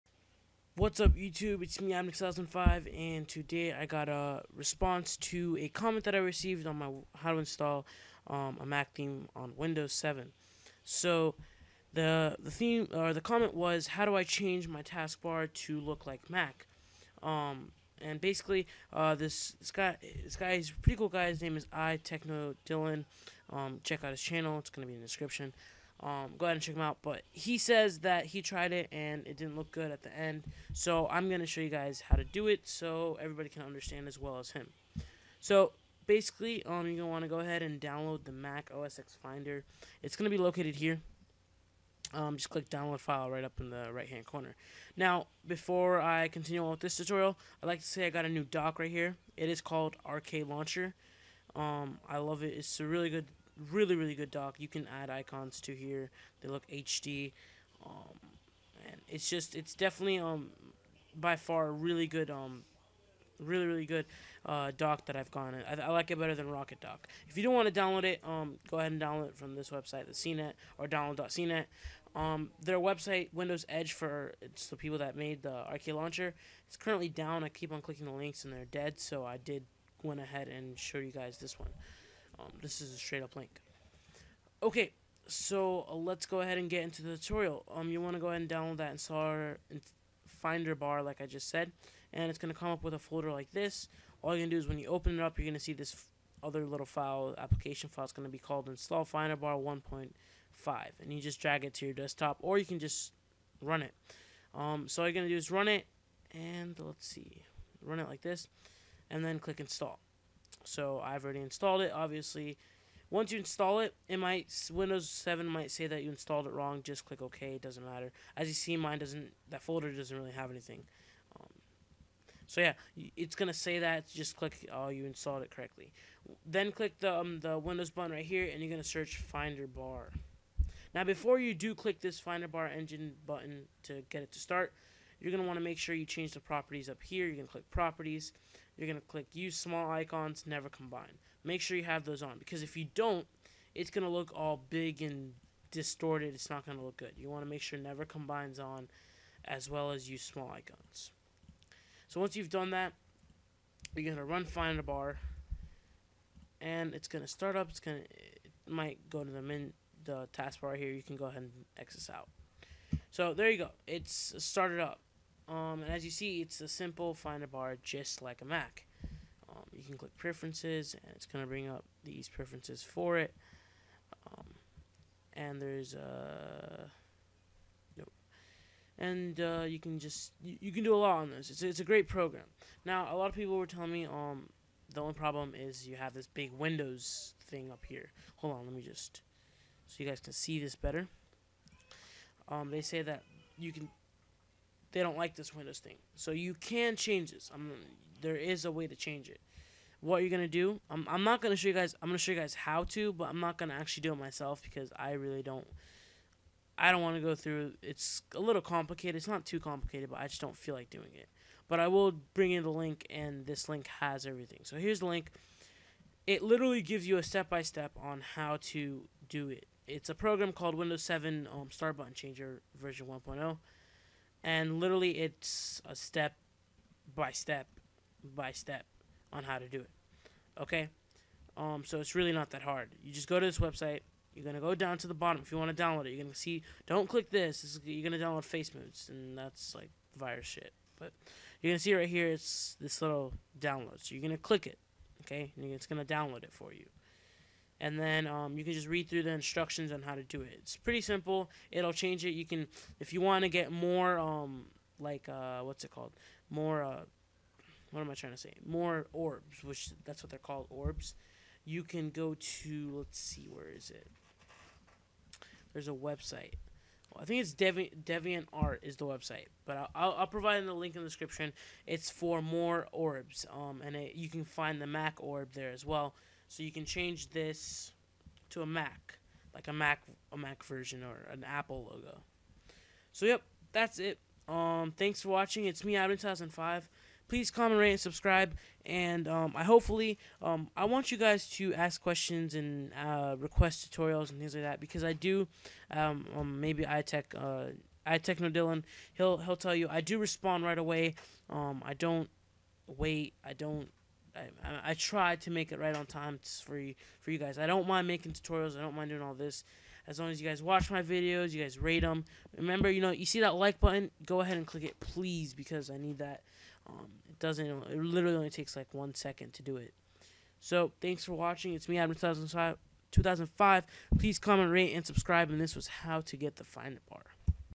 How to get a Mac Taskbar for Windows 7
This a response to a comment left on my How to install a mac theme on windows 7. The comment was left by iTechnoDlyan... Check out his channel.